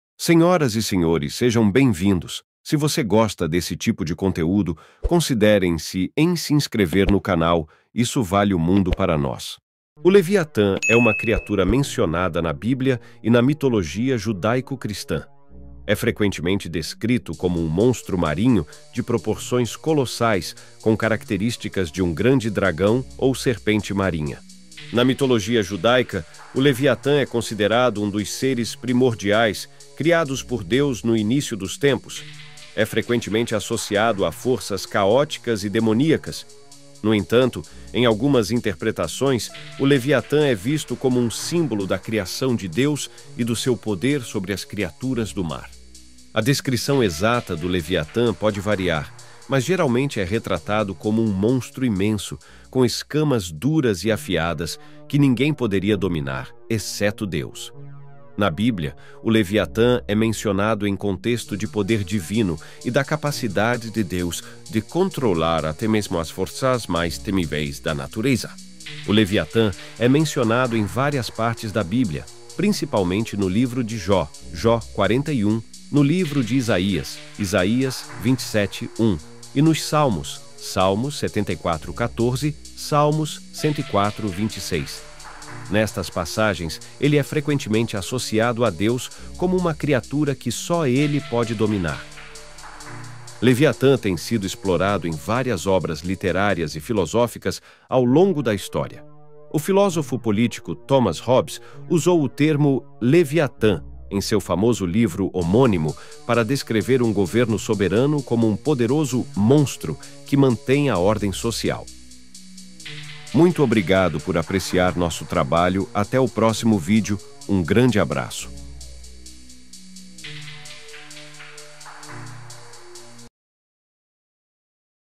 Leviatã o mostro que Deus cita na Bíblia
Leviatã tem sido explorado em várias obras literárias e filosóficas ao longo da história. O filósofo político Thomas Hobbes usou o termo "Leviatã" em seu famoso livro homônimo para descrever um governo soberano como um poderoso "monstro" que mantém a ordem social.

Cultura Popular: O Leviatã também aparece na cultura popular e na literatura contemporânea como uma figura de grande poder e ameaça, muitas vezes associada a criaturas míticas e à fantasia.